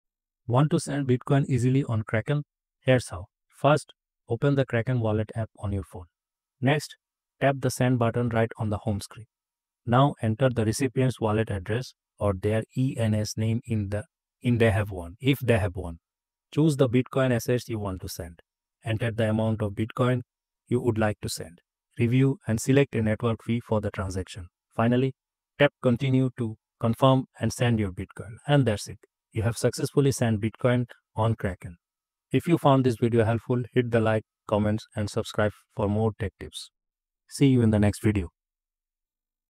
How to Send Bitcoin on Kraken | Transfer Bitcoin From Kraken App To Another Wallet
Want to send Bitcoin on Kraken but not sure how? 🪙 Follow this guide to securely transfer BTC from your Kraken wallet.